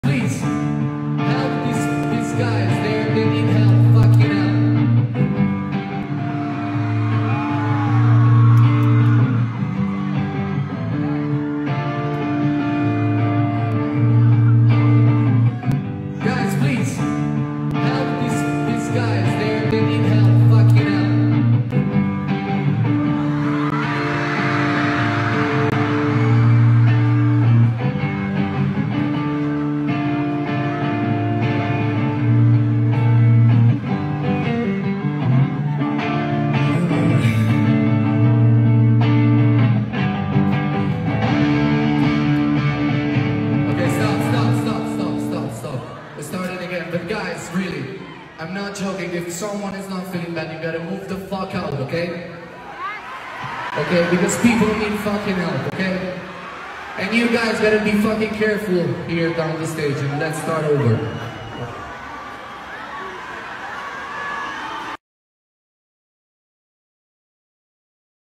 Fan ha un malore, Damiano dei Maneskin ferma il concerto
Durante l'esibizione di Città del Messico, il leader della band romana ha stoppato la musica per consentire i soccorsi